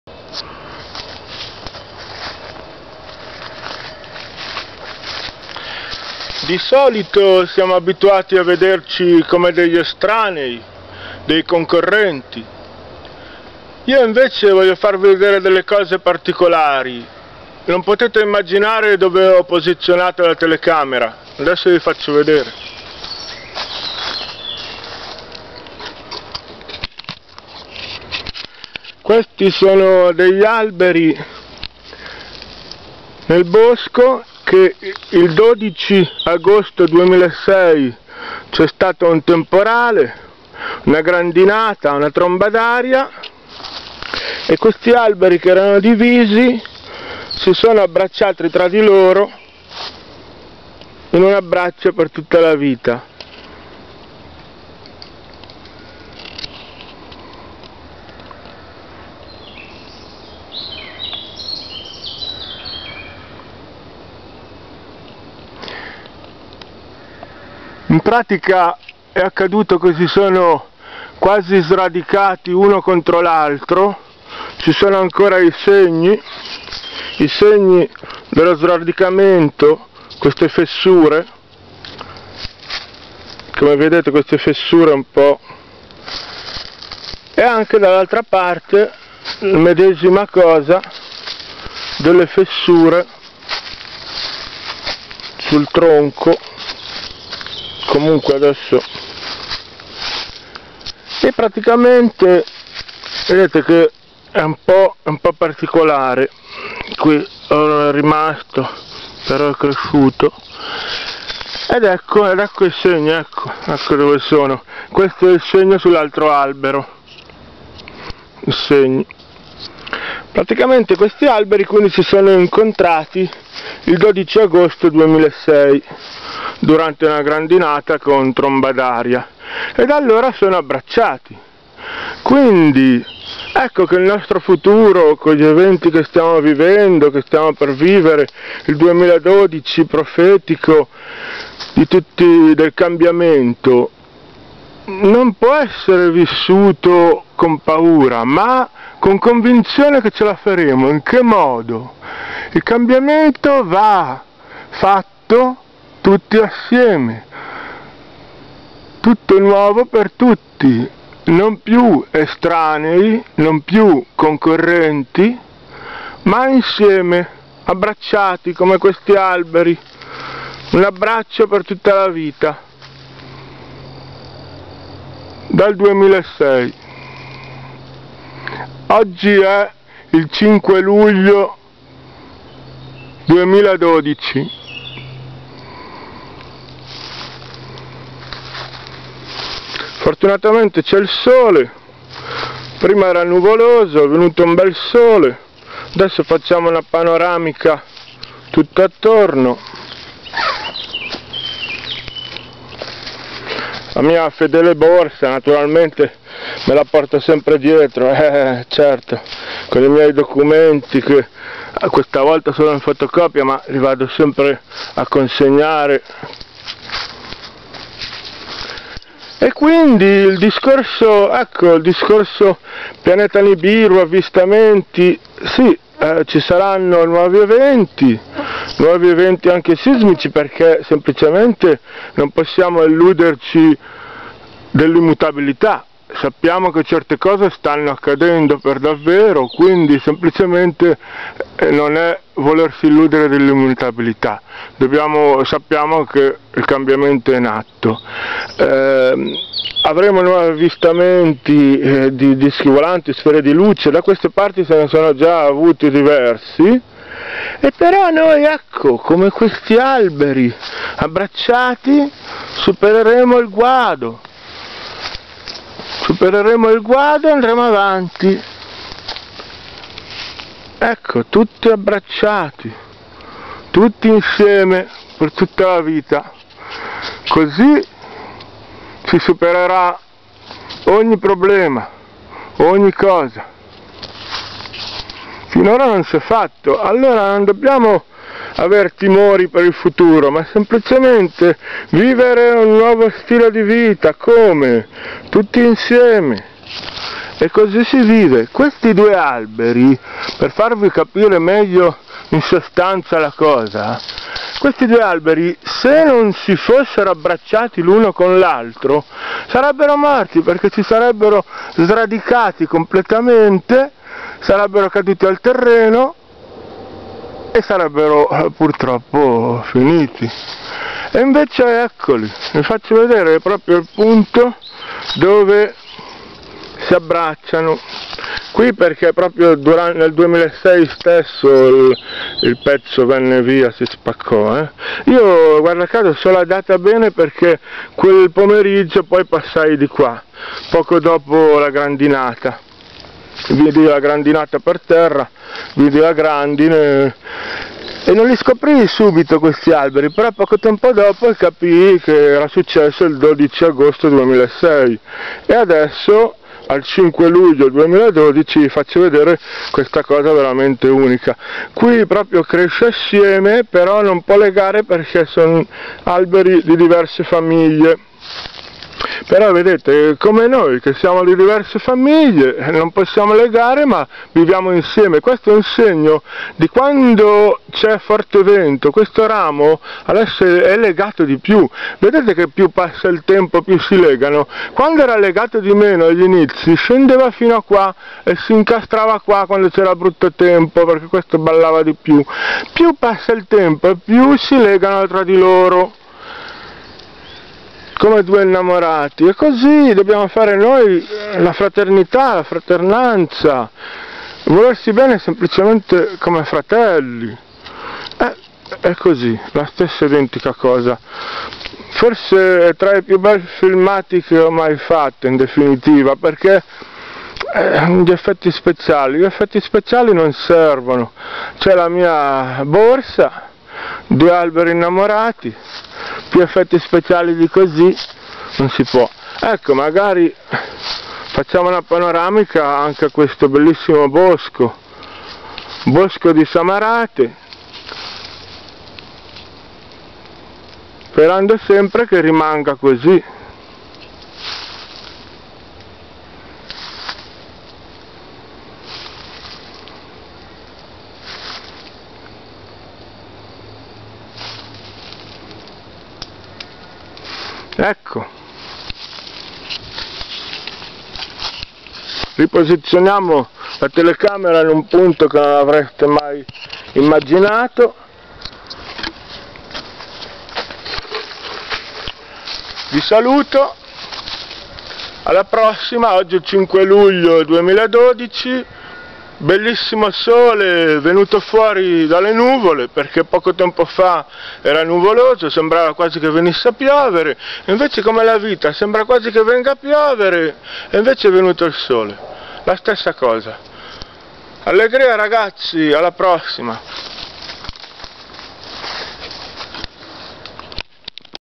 Abbraccio alberi
Sostegno fraterno senza essere estranei o concorrenti. Fraternal support without being outsiders or competitors. . Due alberi vivono abbracciati da quando il 12 agosto 2006 una tromba d'aria, sradicandoli parzialmente, li ha fatti appoggiare l'uno sull'altro, amorevolmente per tutta la vita. Two trees live embraced since August 12, 2006 when a tornado, eradicated partly, made ​​them lean on each other, loving for a life.